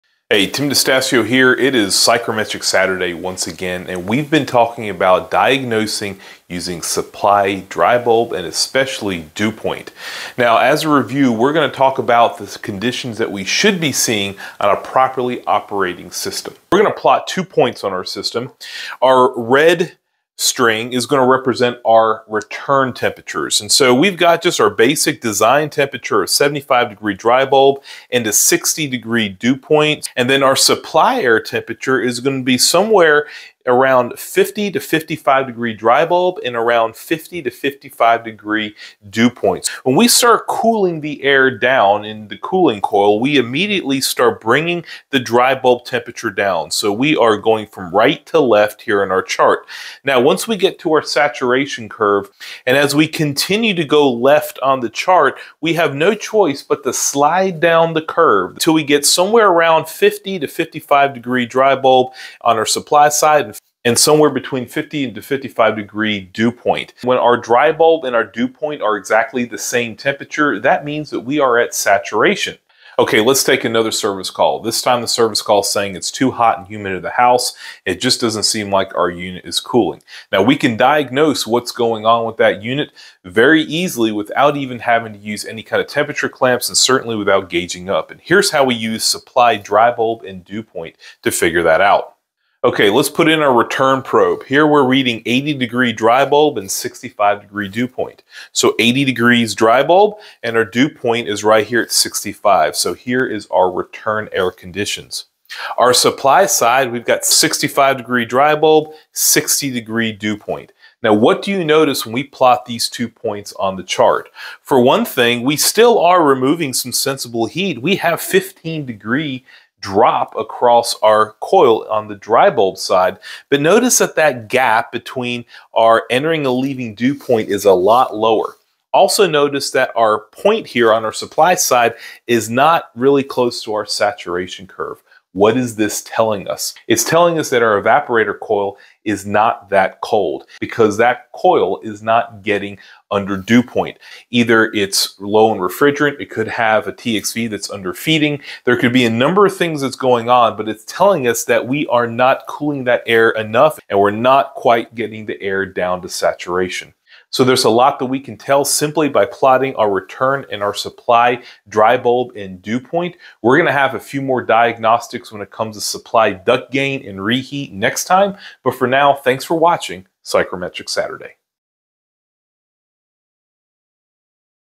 Psychrometric Saturday: Supply Air Diagnostics
PsychrometricSaturday Supply Air Diagnostics. You van tell a lot about how a system runs by just reading supply and return air temperature and dewpoint.